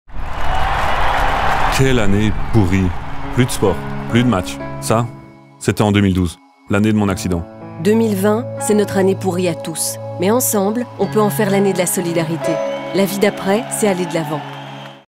Louis: 2012, son année pourrie (campagne de sensibilisation 2020)
L’année 2020 est vraiment une année pourrie. Plus de voyages, plus de sport, plus de fêtes, plus d’Euro, plus de projets…
Mais on peut aussi faire le choix d’avancer. Comme Louis, en chaise roulante depuis un accident de vélo en 2012, et aujourd’hui champion d’aviron et journaliste sportif.

80% des handicaps surviennent au cours de la vie. Quand un accident ou une maladie fait basculer la vie, beaucoup se battent pour aller de l’avant.
Et si on prenait exemple?
2020 ne sera pas l’année des fêtes, des voyages, des spectacles, des projets,… 2020, c’est notre année pourrie à tous. Mais, ensemble, on peut en faire l’année de la solidarité.

La vie d’après, c’est aller de l’avant.
Retrouvez la campagne de CAP48 dans son intégralité sur cap48.be